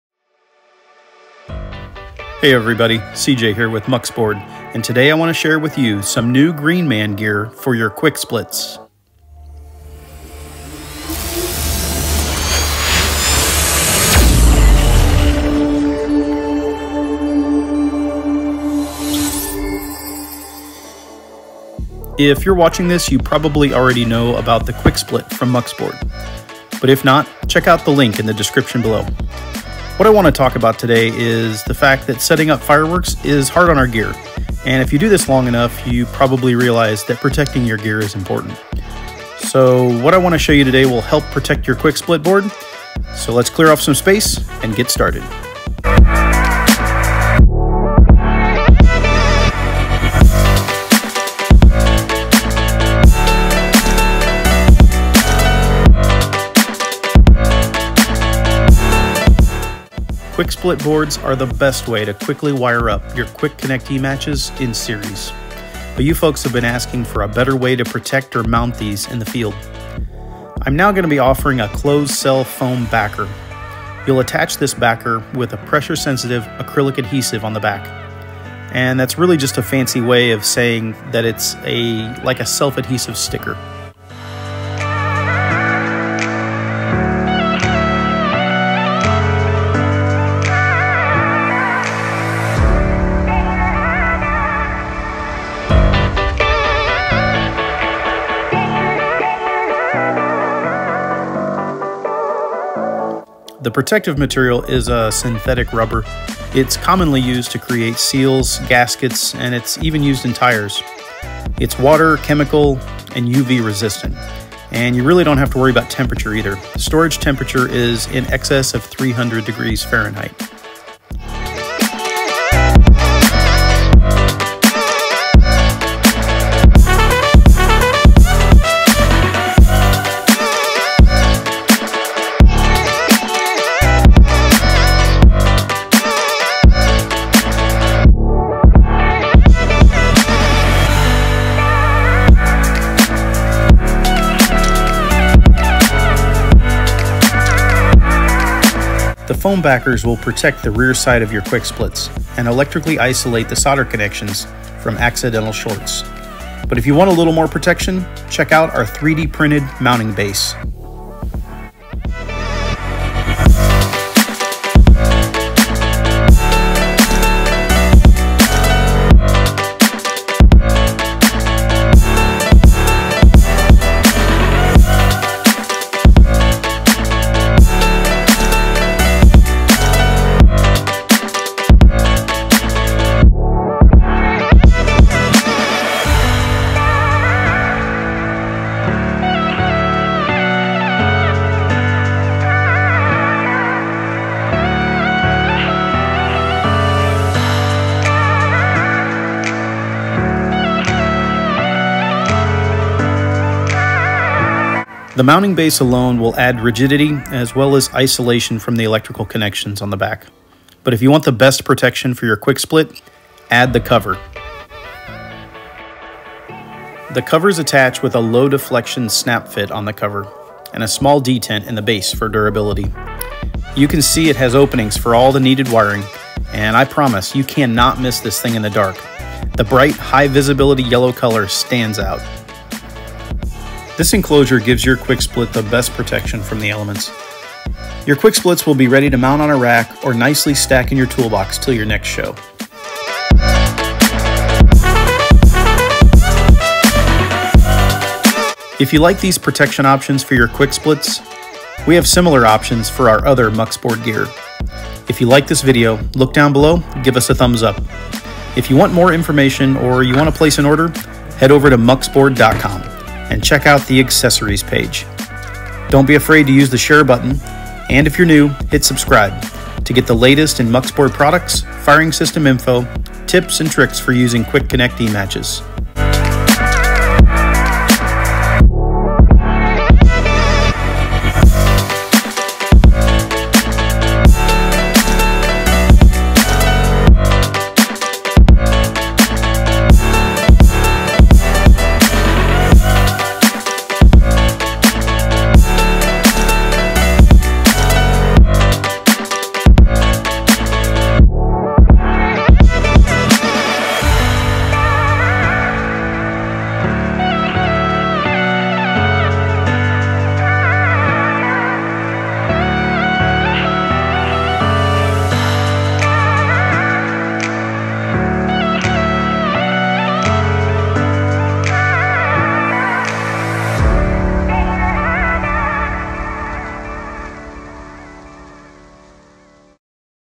Protecting your QuickSplit boards
muuxBRD offers custom QuickPlug / Quick Connect slats, as well as several mounting options.  Protect your slats with a 3D printed mounting base, or a full enclosure.

If you are using Quick Connect ematches, but struggle with series wiring, or avoid it because it’s time consuming check out

If you want more info about mounting bases, enclosures, covers go